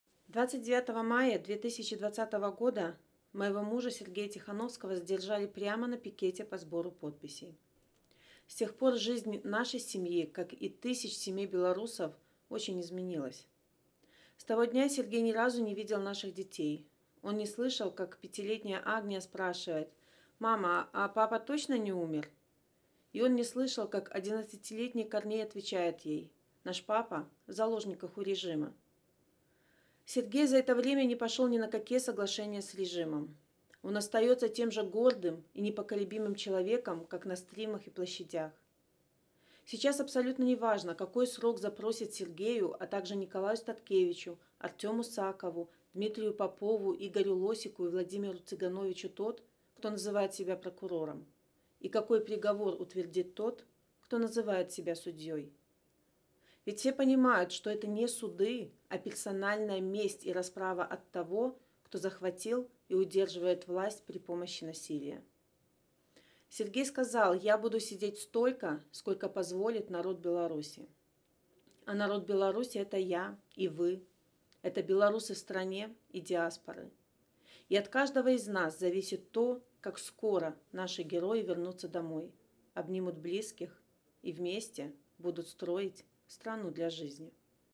«Это месть». Светлана Тихановская – о суде над своим мужем, Сергеем Тихановским
Светлана Тихановская: «29 мая 2020 года моего мужа Сергея Тихановского задержали прямо на пикете по сбору подписей. С тех пор жизнь нашей семьи – как и тысяч семей беларусов – очень изменилась.

С того дня Сергей ни разу не видел наших детей. Он не слышал, как пятилетняя Агния спрашивает: “Мама, а папа точно не умер?” И не слышал, как одиннадцатилетний Корней отвечает ей: “Наш папа – в заложниках у режима”.

Сергей за это время не пошел ни на какие соглашения с режимом. Он остается тем же гордым и непоколебимым человеком, как на стримах и площадях.

Сейчас абсолютно неважно, какой срок запросит Сергею, а также Николаю Статкевичу, Артему САкову, Дмитрию Попову, Игорю Лосику и Владимиру Цыгановичу тот, кто называет себя прокурором, и какой приговор утвердит тот, кто называет себя судьёй. Ведь все понимают, что это не суды, а персональная месть и расправа от того, кто захватил и удерживает власть при помощи насилия.

Сергей сказал: “Я буду сидеть столько, сколько позволит народ Беларуси”. Народ Беларуси – это я и вы, это беларусы в стране и из диаспоры. И от каждого из нас зависит то, как скоро наши герои вернутся домой, обнимут близких и вместе будут строить страну для жизни».